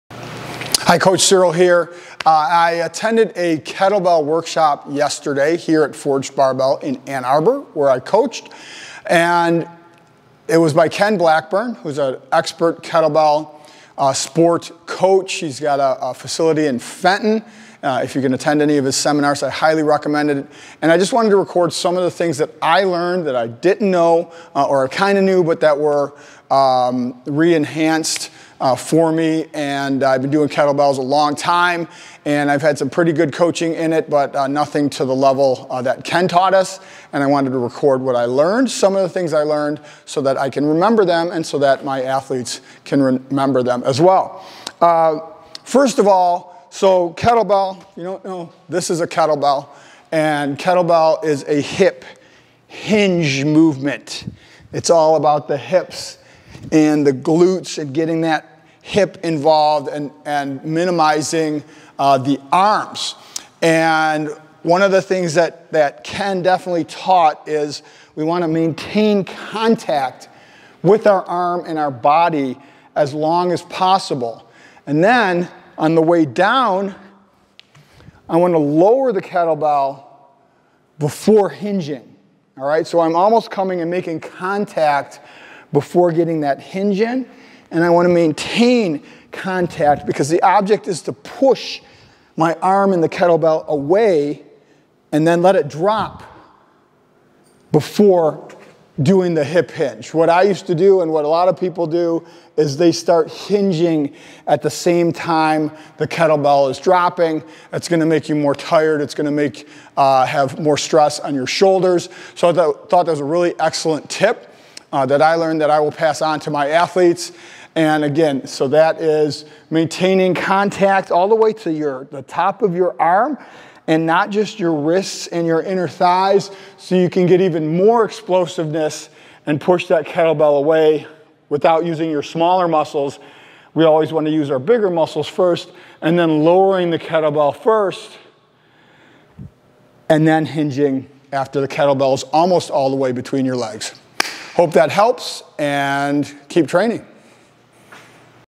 Proper Kettlebell Technique
Coach Cyril talks about what he learned at a recent Kettlebell workshop he attended.